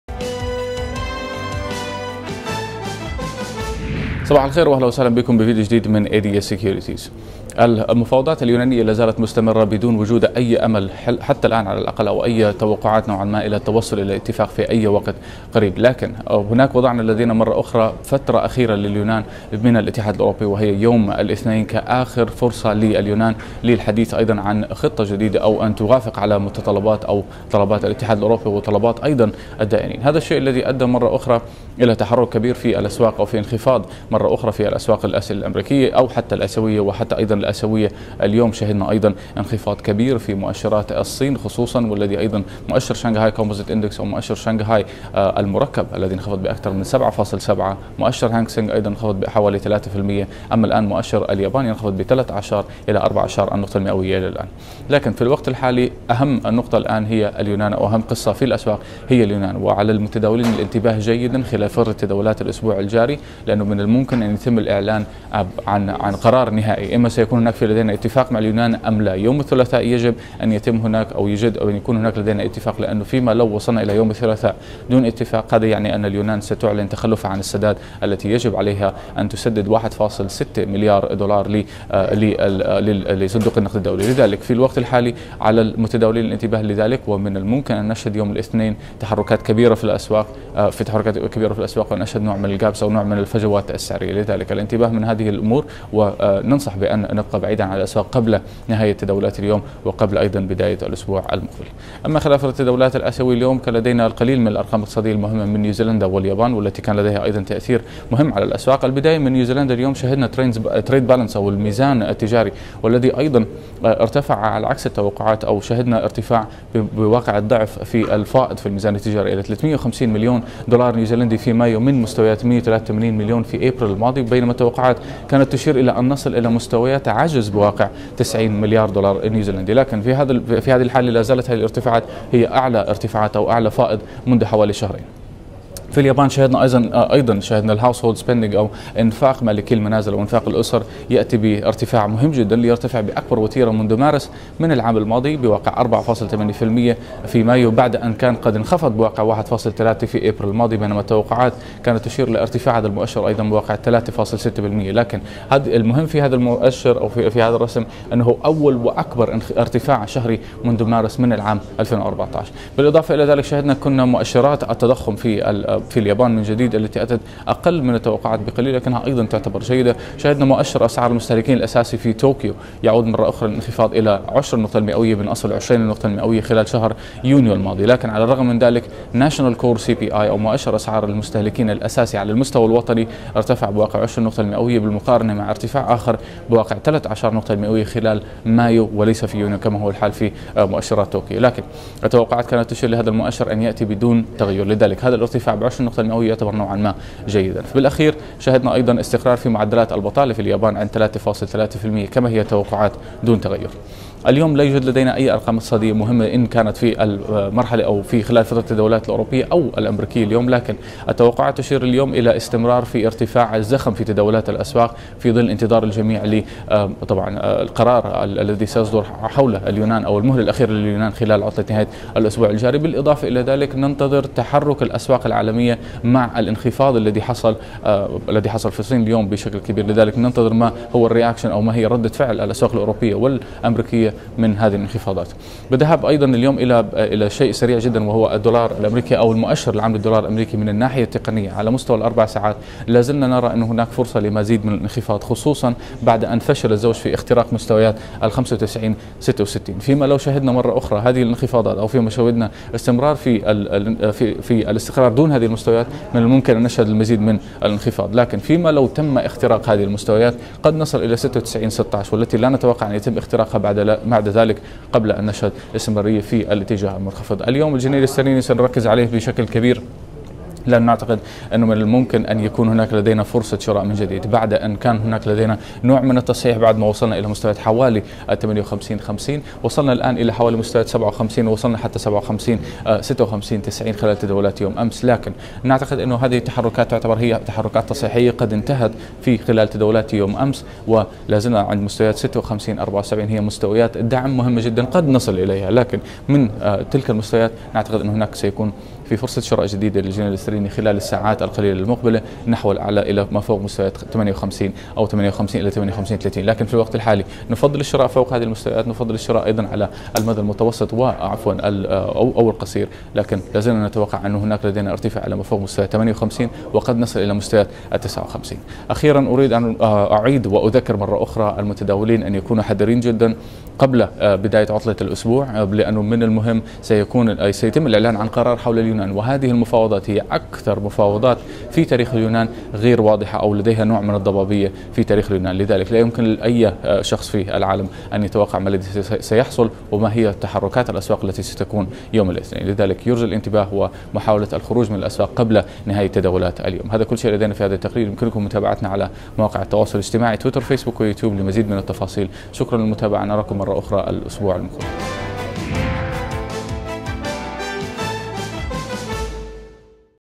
الاسواق تنتظر اكثر اسبوع خطراً في تاريخ اليونان
الاسواق العالمية بانتظار اهم واخطر عطلة نهاية اسبوع لم نشهد لها مثيل منذ الازمة المالية العالمية الى الان, حيث ان المهلة الاخيرة لليونان اصبحت حتى يوم الاحد المقبل. في حال تم الاعلان عن اتفاق او لم يتم التوصل الى اتفاق , في الحالتين الاسواق ستتفاعل مع القرار بشكل واضح وكبير ايضاً. نلقي نظرة على السيناريوهات المتوقعة لهذا الاجتماع وما هو القرار المناسب لكل المتداولين قبل نهاية الاسبوع الجاري. كما نلقي نظرة ايضاً على الارقام الاقتصادية التي تم الاعلان عنها خلال فترة التداولات الاسيوية من اليابان ونيوزلندا والتي تتضمن ارقام الميزان التجاري في نيوزلندا وارقام التضخم وانفاق الاسر ومعدلات البطالة في اليابان. اخيراً, نلقي نظرة على  تحركات المؤشر العام للدولار الامريكي والجنيه الاسترليني مقابل الدولار من الناحية التقنية وتوقعات الفترة القصيرة المقبلة.